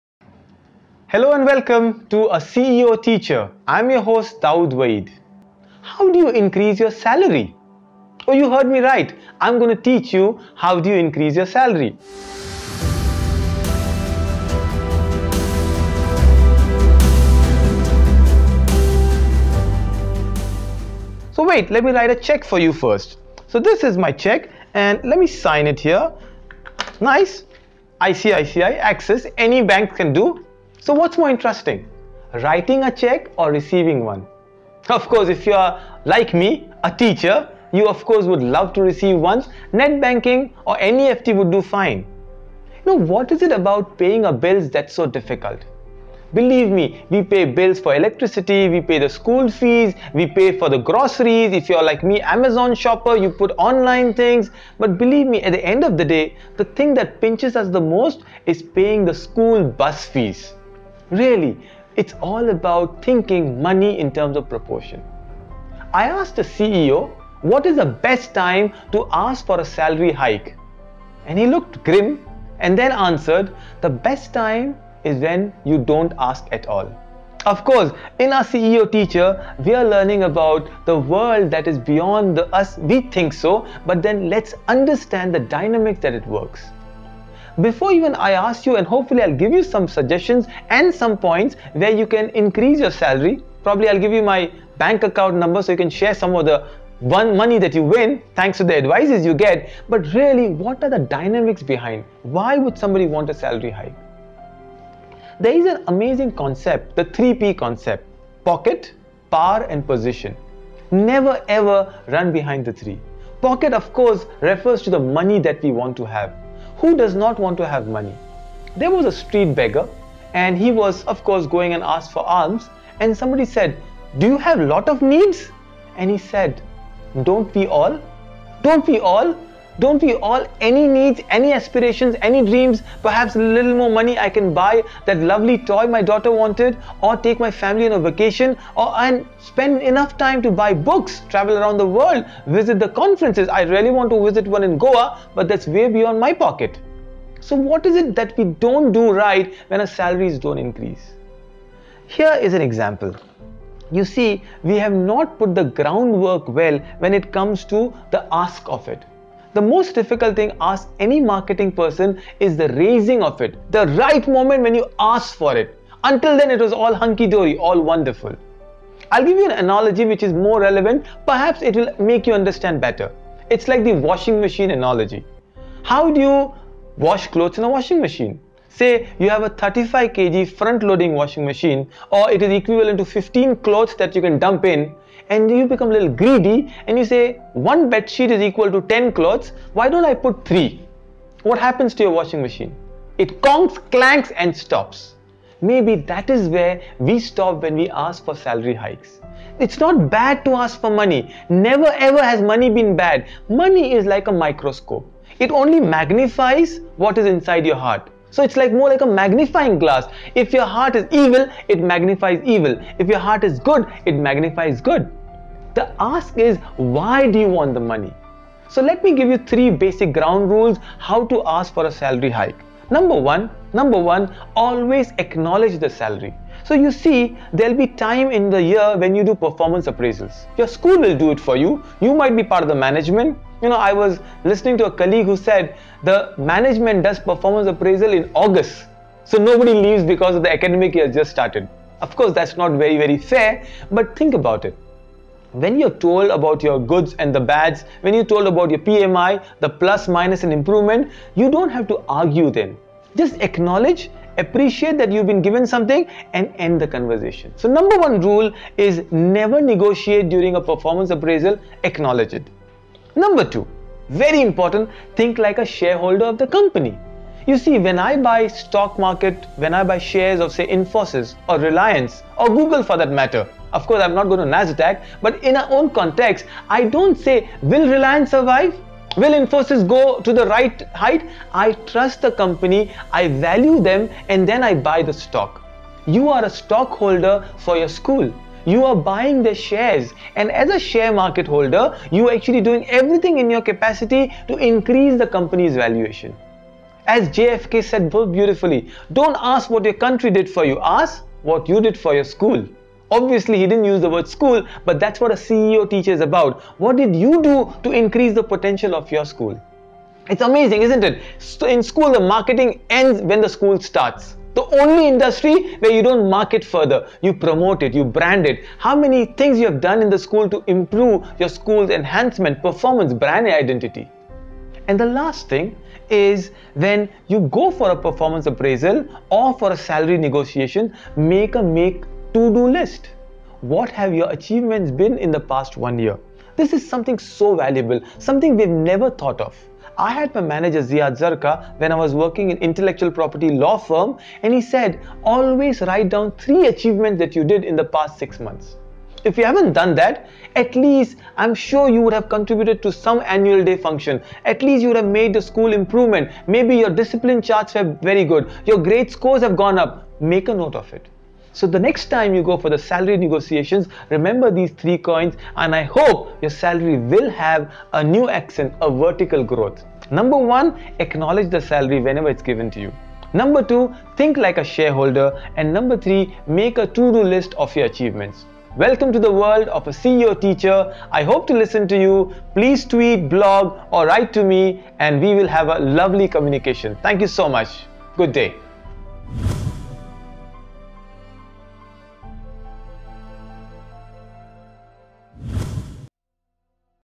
A CEO Teacher | Episode 03 | How to increase your salary? | Dawood Vaid
A CEO Teacher

What is more difficult --- Writing a cheque or receiving one? Of course, writing one.  And it become even more difficult when we ought to pay the electricity bill or the most difficult one - the school bus fees!

Money – a vital need

Money is an important parameter in everyone's life. Money is necessary not only a luxury. We earn, spend, save, demand. Demand? Is it the salary time? How much can be expect the hike? What would the bonus be? There are innumerable questions that we seeks as salary is expected around the year end performance appraisal time. In-fact one institution I am aware of, has its annual performance appraisal in August, which is the beginning of the year. How mean, I thought to myself. This must be to ensure that the teachers can't leave or seek another job, as the academic year has begun!

So, what is the best time to ask a pay hike? A CEO replied, best time is DON'T ASK. Well, we won’t take that for an answer. We will think of a strategy. I honestly agree that asking for a pay hike is an art. If we demand too soon, it might be counter-productive. It's like overloading a Washing Machine. We all are overloaded, burdened by our own expectations. So remember the three P of money:

Pocket
Position
Power

3 P Questions

Ask yourself, what is it you are seeking. Are you in a 'position' to seek the hike? Do the one you seek have the 'power' to grant your request? Does the current 'pocket' or 'purse' has such budgets to approve your salary? Equipped with these answers, now arm yourself with some key pointers. When a salary is given to you, do not negotiate. Accept it at the moment. Also plan your hike like a shareholder. Think of the current situation of the institution and the timing of your ask. Like JKF said, "Don't ask what your country did you for, ask what you did for your country." List down your achievements over the past year and then move ahead confidently for the ask.

And yes, if you the desired hike, count me in for the party!

Three Lessons

Acknowledge the salary [never negotiate - accept and thank]
Think like a shareholder [you don’t ask Google when buying the share - you think long term]
Make a to do list [what have you achieved over the past year; be clear]